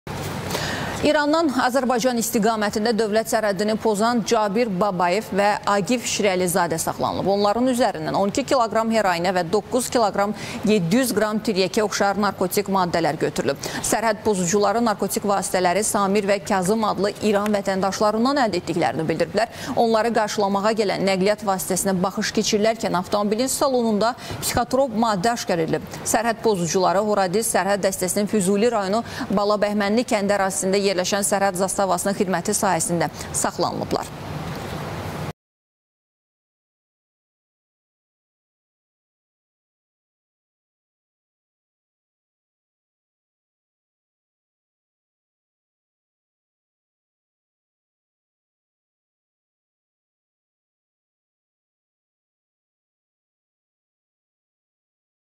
Sərhədi pozan şəxslər saxlanılıb
İrandan Azərbaycan  istiqamətində dövlət sərhədini pozan Cabir Babayev və Aqif Şirəlizadə saxlanılıb. Onların üzərindən 12 kq heroinə və 9 kq 700 qram tiryəkə oxşar narkotik maddələr götürülüb. Sərhəd pozucu¬ları narkotik vasitələri Samir və Kazım adlı İran vətəndaşlarından əldə etdik¬lərini bildiriblər. Onları qarşılamağa gələn Nəqliyyat vasitəsinə baxış keçirilərkən avtomobilin salonunda psixotrop maddə aşkar edilib. Sərhəd pozucuları “Horadiz” sərhəd dəs¬təsinin Füzuli rayonu Bala Bəhmənli kəndi ərazisində yerləşən sərhəd zasta-vasının xidməti sahəsində saxlanılıblar.